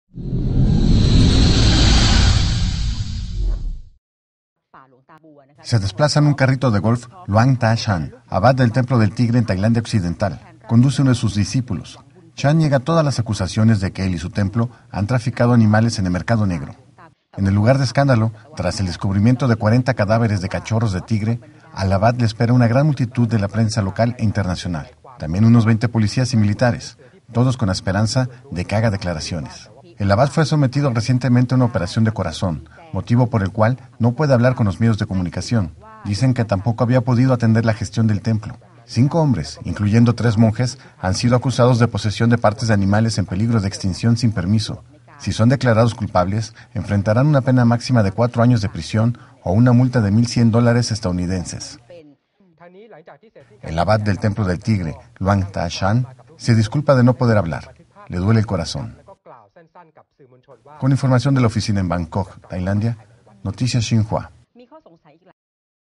Niega abad del Templo del Tigre traficar animales
Luang Ta Chan, abad del Templo del Tigre en Tailandia occidental, niega todas las acusaciones de que él y su templo han traficado animales comerciales en el mercado negro.